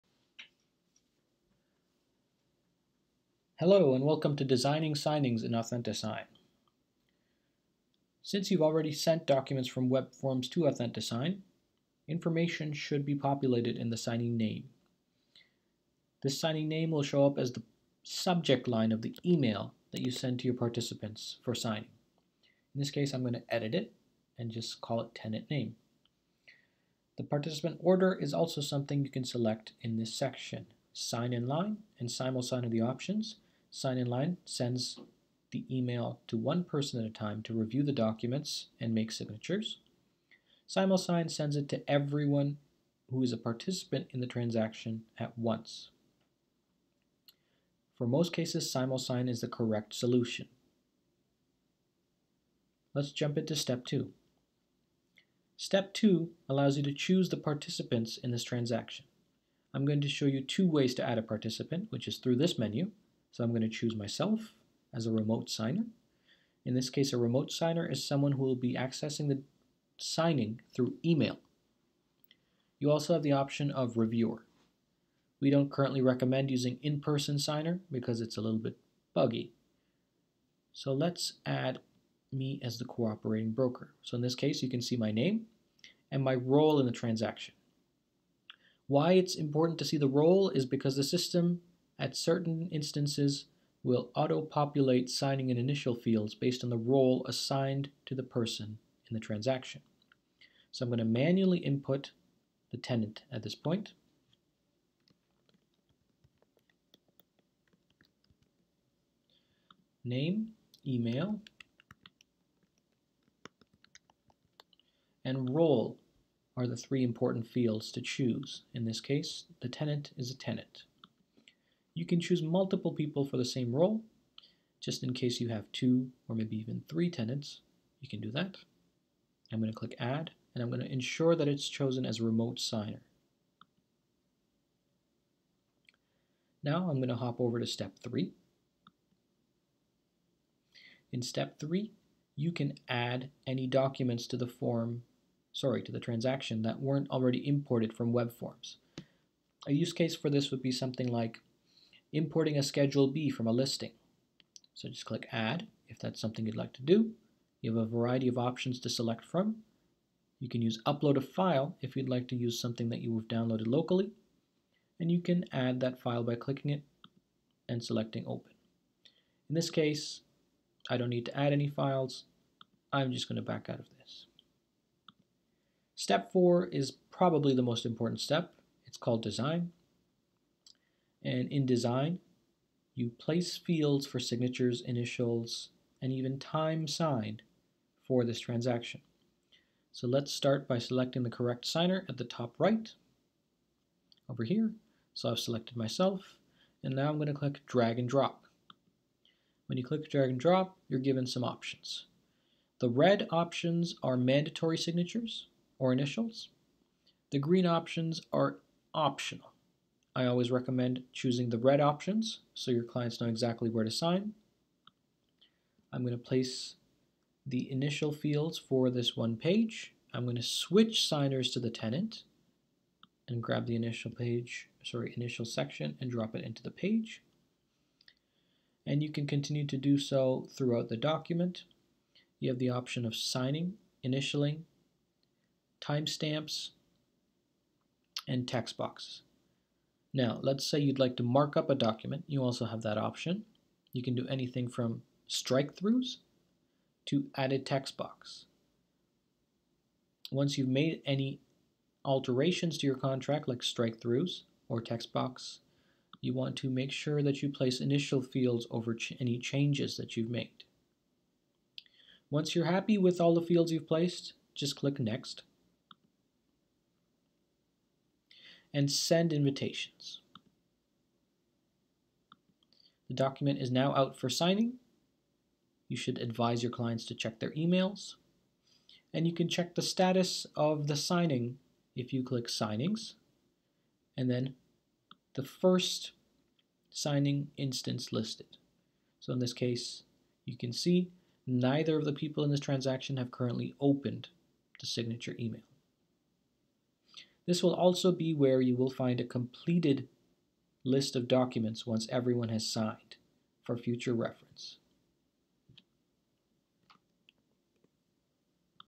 Designing Signings
In this video, learn how to design signings for any transaction documents sent to Authentisign.